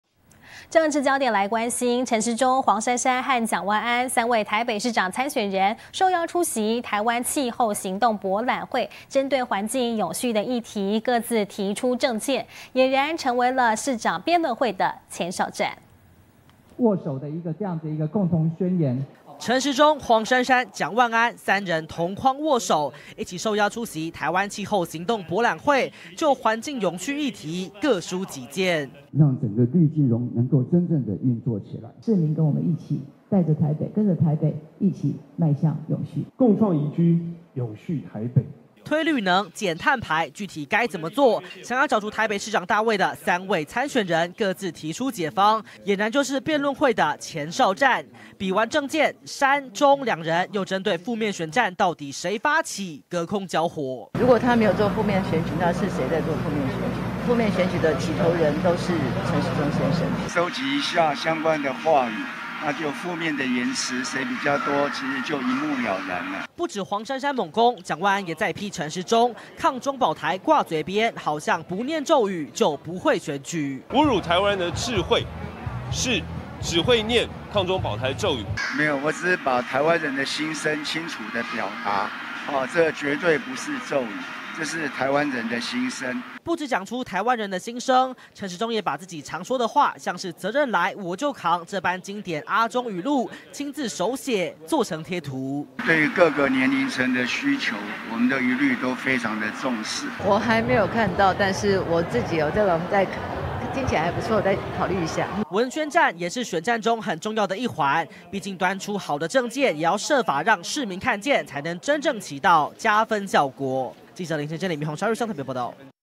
北市辯論前哨戰 中.珊.蔣各提環境永續政策
陳時中、黃珊珊和蔣萬安，三位台北市長參選人受邀出席「台灣氣候行動博覽會」，針對環境永續議題，各自提出政見，儼然就是市長辯論會的「前哨戰」。 而在時事議題上，蔣萬安受訪時再轟陳時中，只會唸「抗中保台」的咒語，陳時中回應，自己是講出台灣人的心聲。而陳時中也和黃珊珊，針對「負面選戰」議題隔空交火。